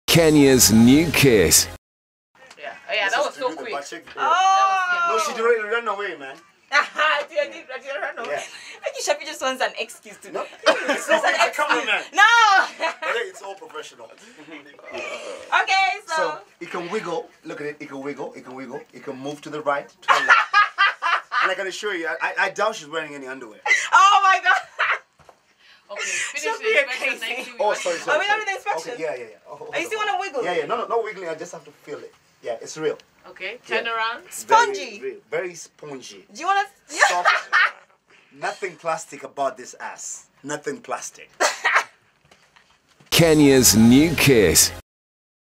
Hilarious Video! Shaffie Weru Fondles Vera Sidika's Delicious Bum
Kiss FM presenter Shaffie Weru fondles Vera Sidika's bum